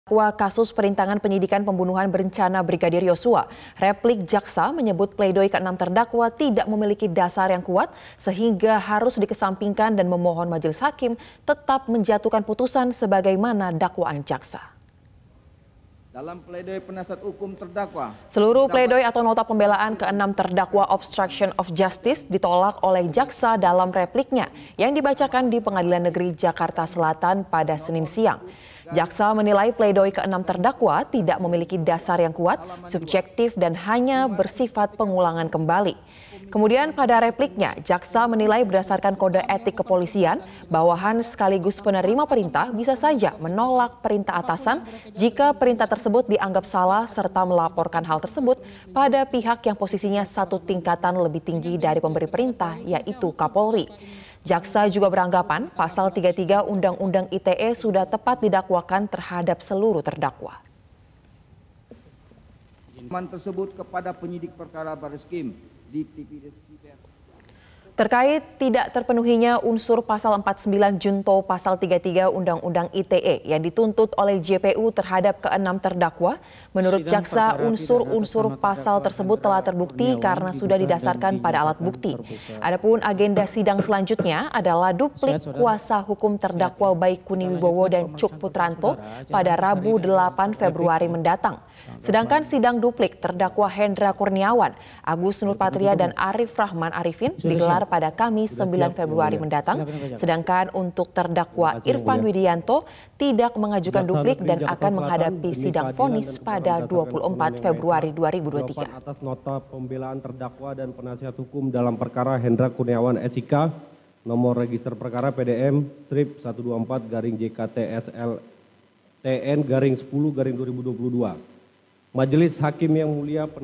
Jaksa Tolak Semua Pledoi Anak Buah Sambo
MetroTV, Kasus perintangan penyidikan pembunuhan berencana Brigadir Yosua Hutabarat. Replik jaksa menyebut keenam terdakwa tidak memiliki dasar yang kuat sehingga harus dikesampingkan dan memohon majelis hakim tetap memutuskan sebagaimana dakwaan jaksa.